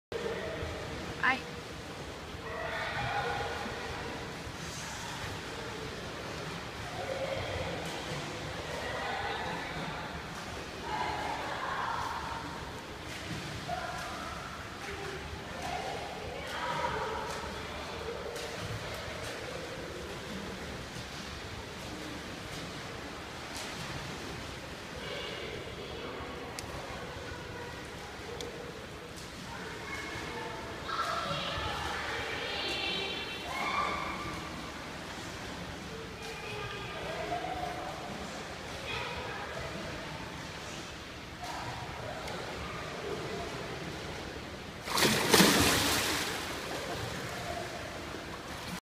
ALS, also DPB challenge
Thanks to William Huang for the nomination.
I hope that ALS will be a curable decease someday.
This time, also I would like you to know another incurable decease, DPB.
My best friend, he used to have DPB. He couldn't breathe by himself because his lungs didn't work less than 50% comparing with ordinary people. Only one solution he can survive was a transplant. So he got 36 hours surgery to replace his whole lungs to new ones donated from his father and brother. Now he lives with his beautiful wife and two cute kids. But, he has to take ridiculously large quantities of medicines everyday in his entire life.
There are many incurable deceases in the world and we don't know most of them. I think this campaign is good opportunity to know, learn and study them.
By the way, I didn't want to waste ice and water, so, instead, I jumped in the water!